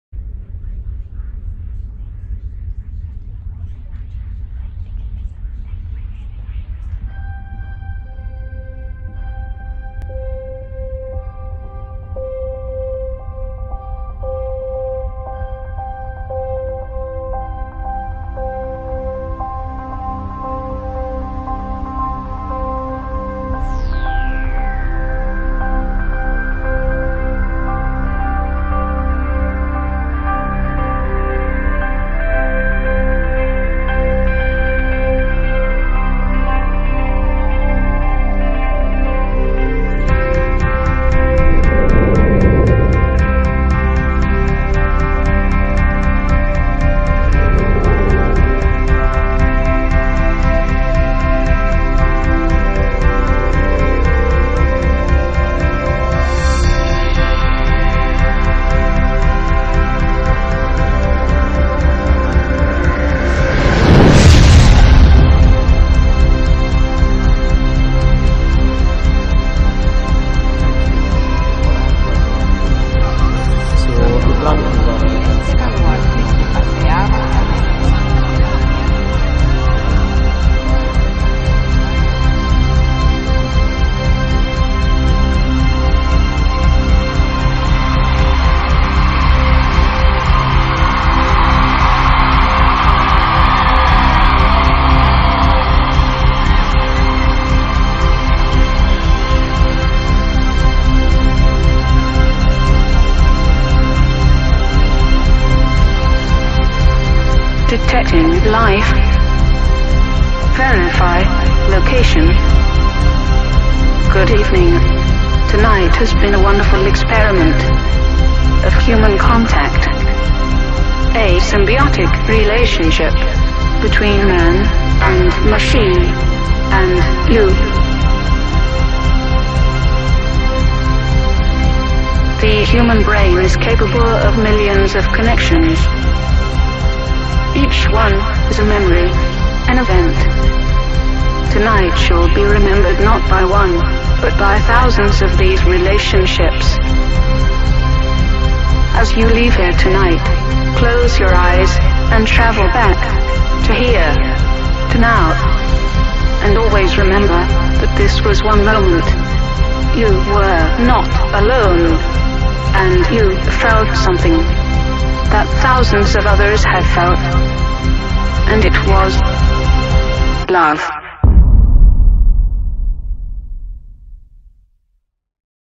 The ending of Love
An astronaut finds comfort in the historic and emotional story of recently passed human beings and their personal experiences of connection and sentiment after being isolated in space as the lone survivor of earth.